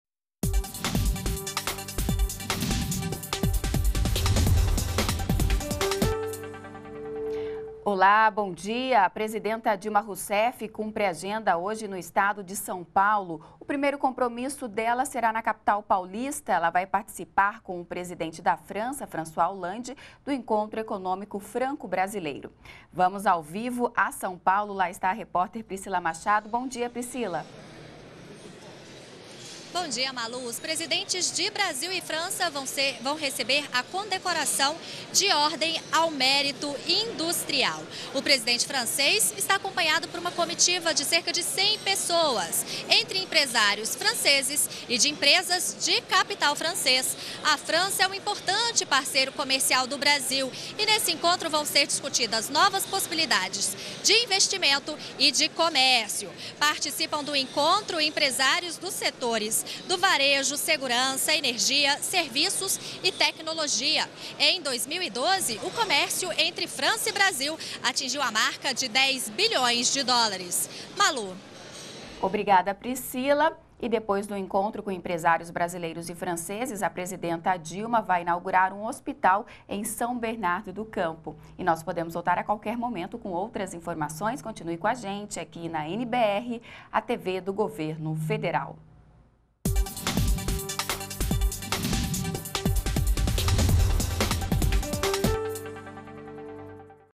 Presidentes de Brasil e França se reúnem com empresários em São Paulo
ATIVIDADES DA PRESIDENTA - 13.12.13: Nesta sexta-feira (13), a presidenta Dilma Rousseff cumpre agenda em São Paulo. No primeiro compromisso, Dilma participa do Encontro Econômico Franco-Brasileiro junto com o presidente francês, François Hollande, e com empresários brasileiros e franceses de diversos setores. Na ocasião, os presidentes receberão a condecoração de Ordem ao Mérito Industrial. A França é um importante parceiro comercial do Brasil, neste encontro serão discutidas novas possibilidades de investimento e de comércio. Após esse compromisso, a presidenta participa da inauguração do Hospital das Clínicas Municipais José Alencar.